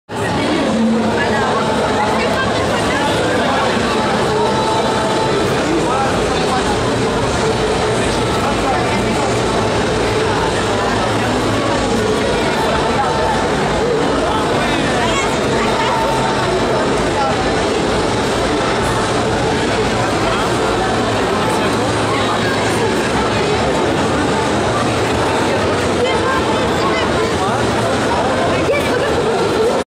Mangame Show Fréjus 2016 - Samedi - bonus dédicace de David Lafarge Pokémon et MissJirachi
Séance dédicace de David Lafarge Pokémon et  MissJirachi au Mangame Show Fréjus 2016
retrouvez les :